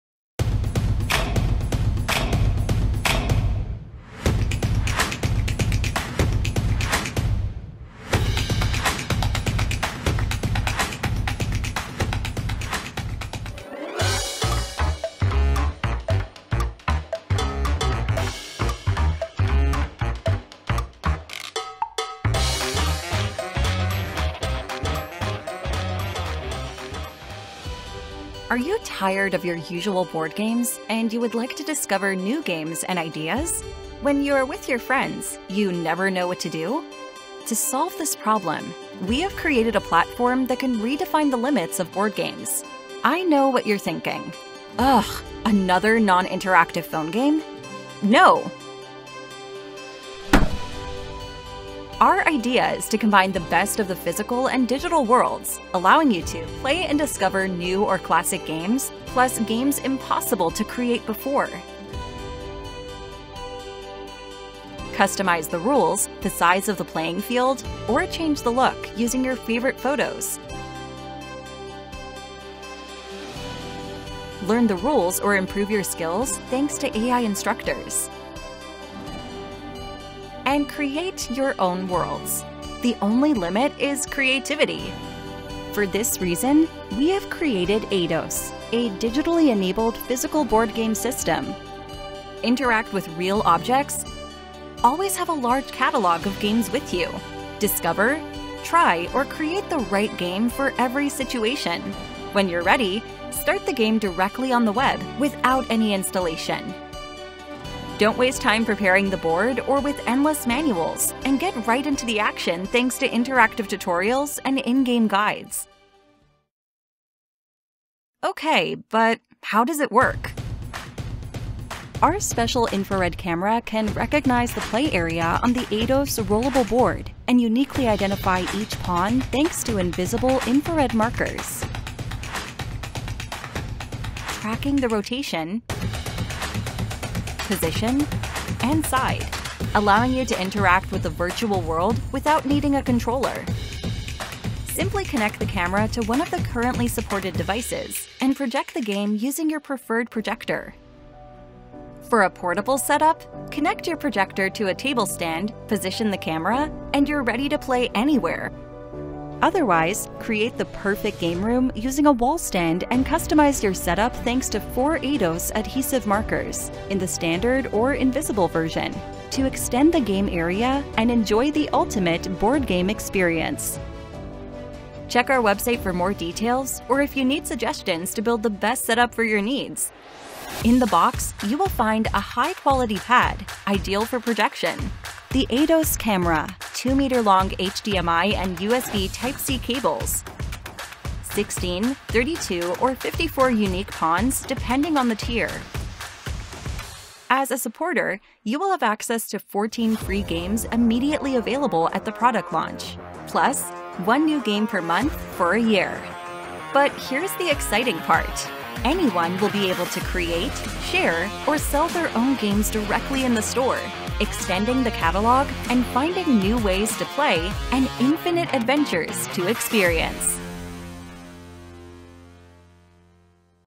Eidos: Digitally enabled physical board games platform - trailer
Physical board and pawns system that unlocks the doors to an endless games hub, combining the best of the digital and physical world.